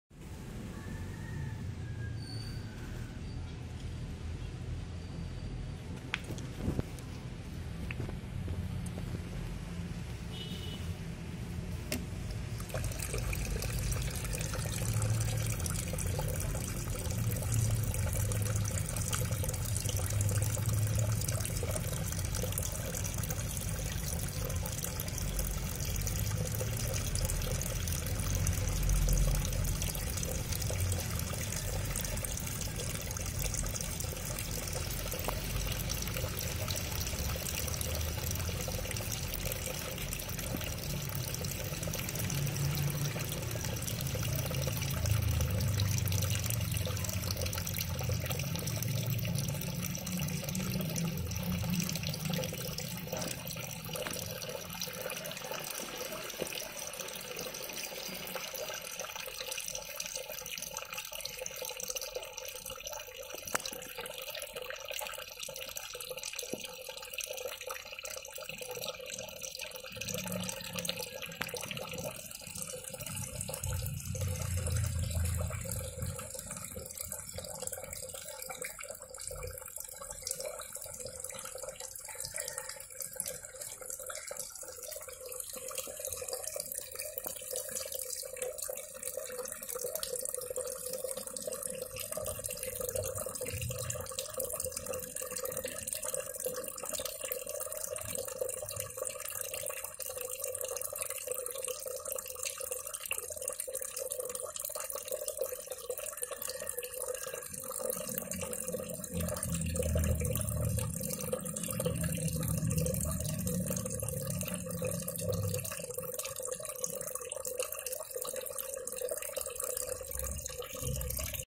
Adding old stock water to my guppy pond
This is how I add old stock water to my guppy pond.